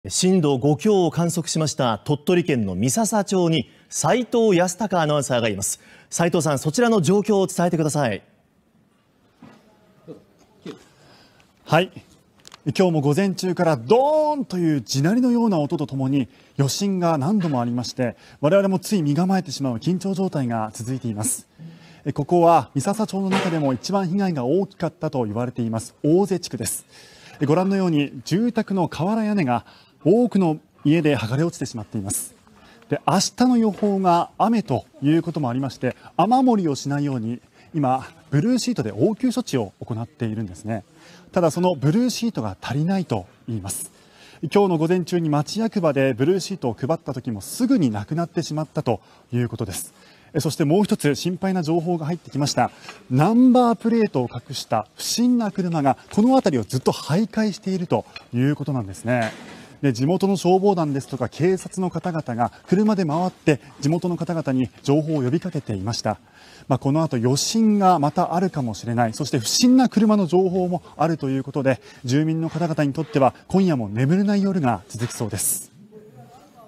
続く余震に不審車両の目撃情報も 住民に不安広がる(16/10/22)
鳥取県で最大震度6弱を観測した地震から一夜、住民が避難した住宅街では心配な情報も寄せられています。震度5強を観測した鳥取県三朝町から報告です。